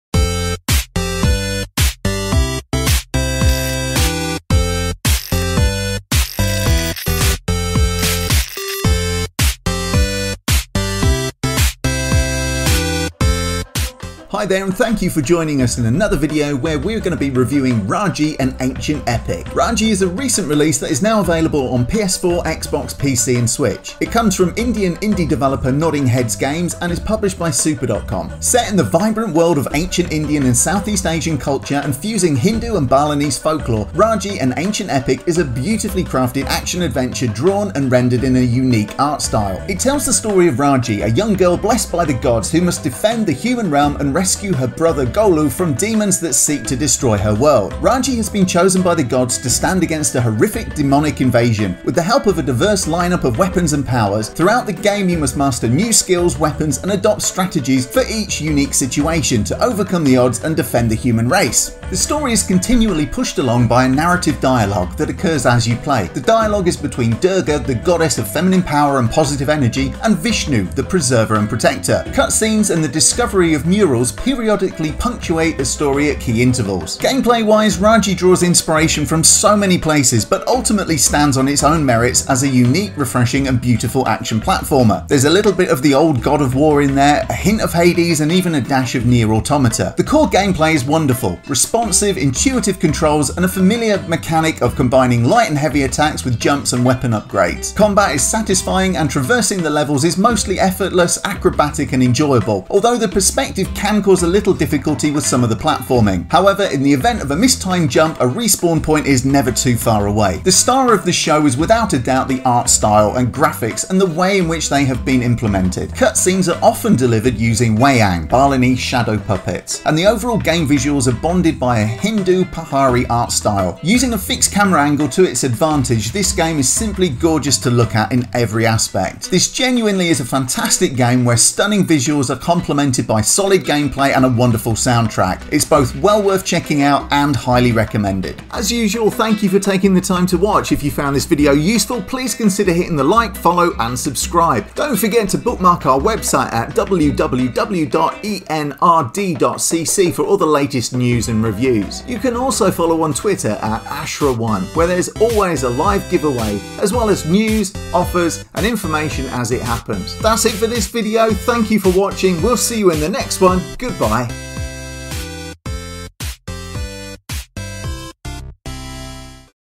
Raji: An Ancient Epic review
A quick video review for gorgeous action platformer, "Raji: An Ancient Epic".
This stunning game comes from Indian indie developer Nodding Heads Games and is available now on PS4, Xbox, PC and Switch.

follow @Aashraa1 on Twitter where there is always a free game giveaway on the go!